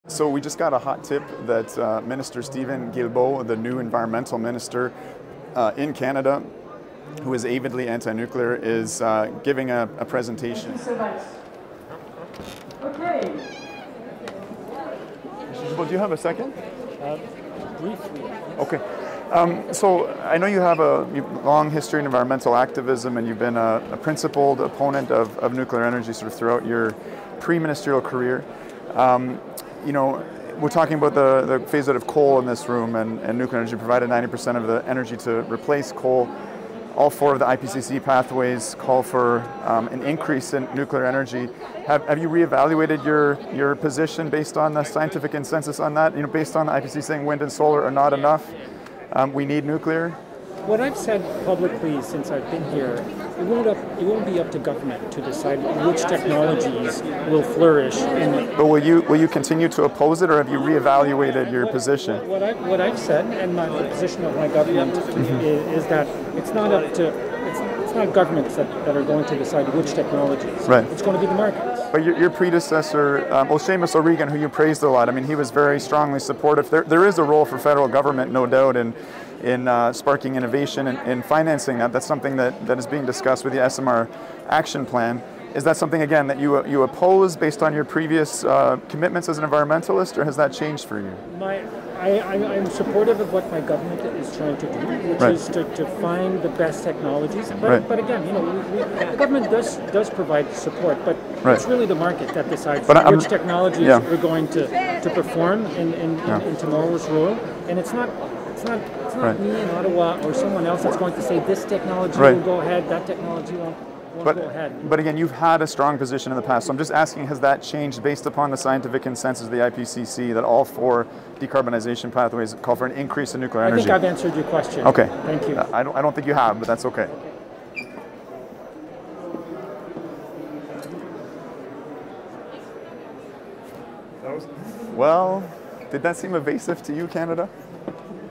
COP26: Environment Minister Guilbeault Confronted on Nuclear Energy
President of Canadians for Nuclear Energy, Dr. Chris Keefer, questions new Minister of Environment and Climate Change, Steven Guilbeault, on his previous life as an anti-nuclear activist. Dr. Keefer asks the minister whether his position has changed given that all four of the principal IPCC decarbonization pathways call for an increase in nuclear energy to limit global warming to 1.5 degrees.

His answer is considerably less clear than the science.